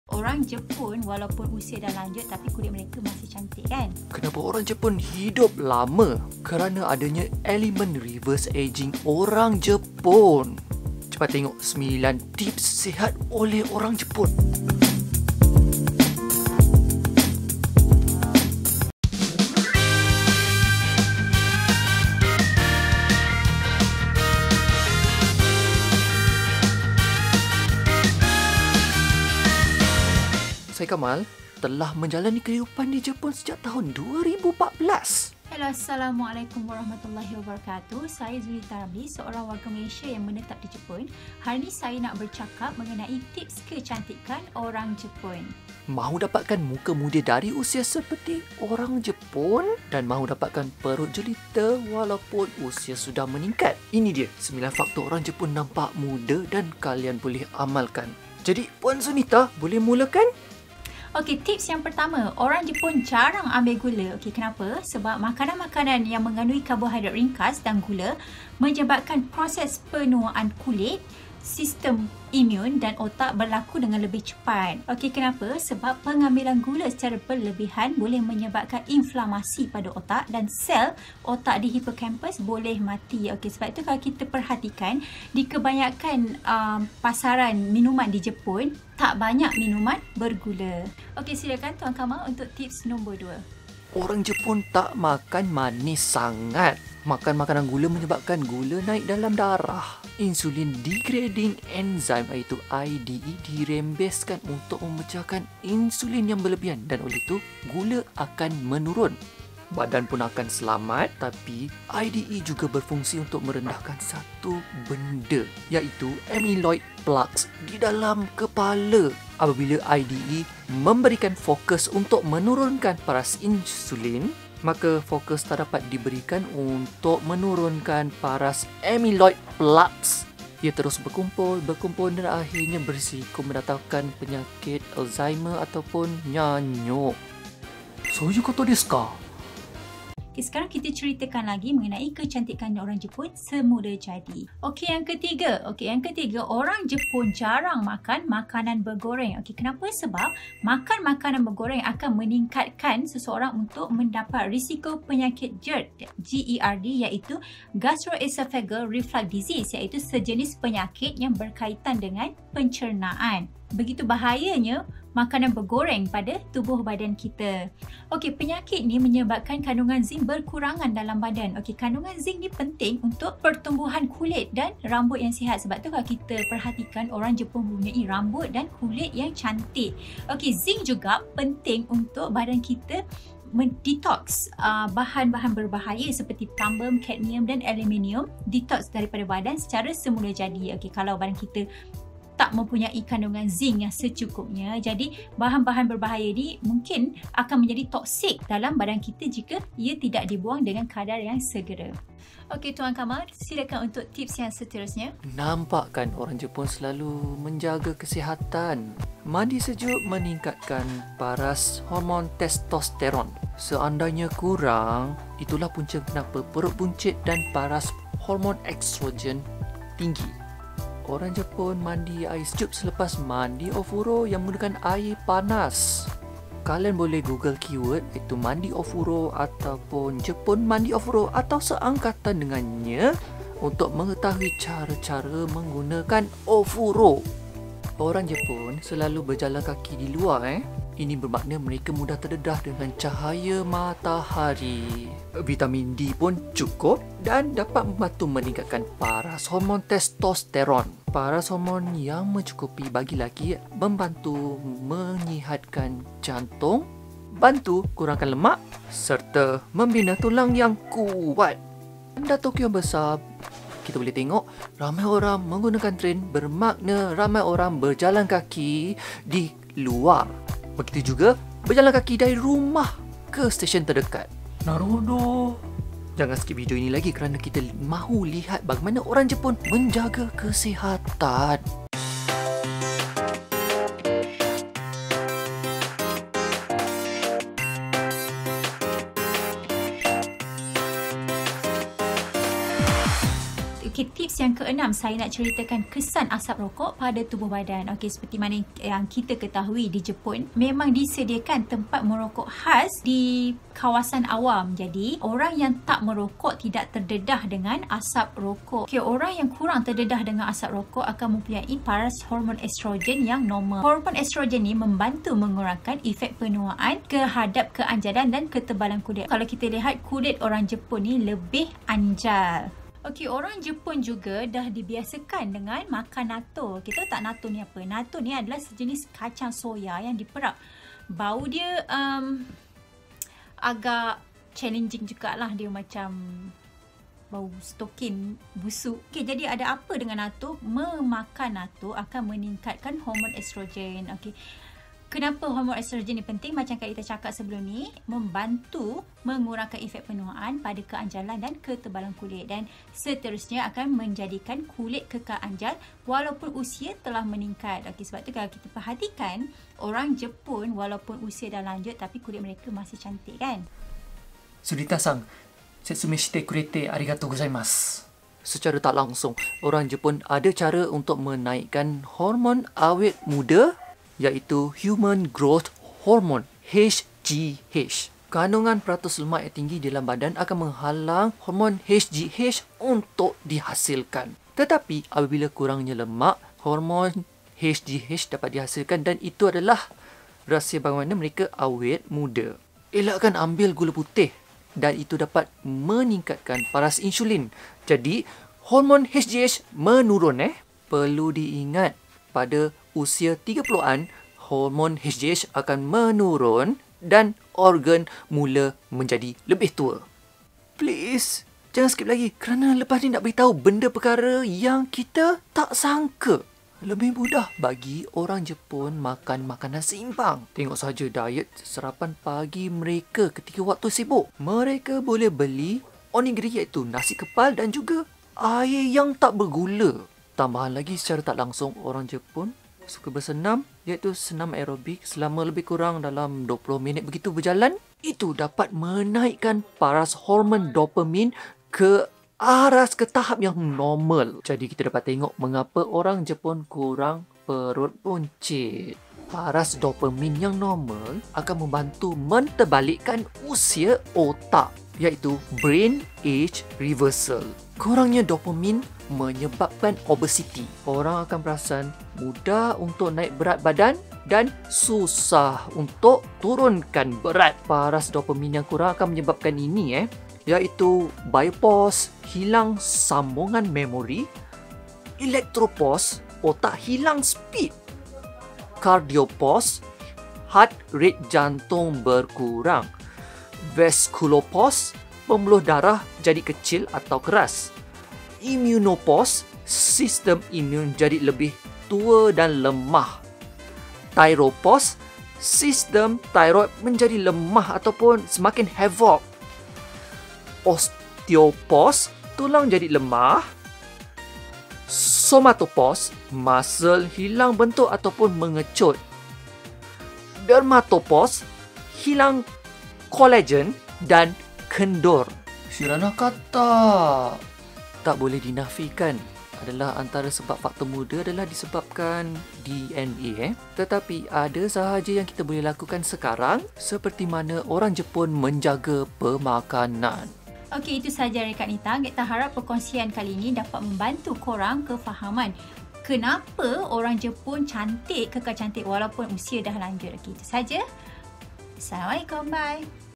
9 Tips Kecantikan Orang Jepun
Kenapa orang Jepun walaupun sudah lanjut usia pun masih kelihatan muda? Video ini memaparkan rahsia bagaimana orang Jepun boleh nampak kekal muda walaupun usia sudah lanjut.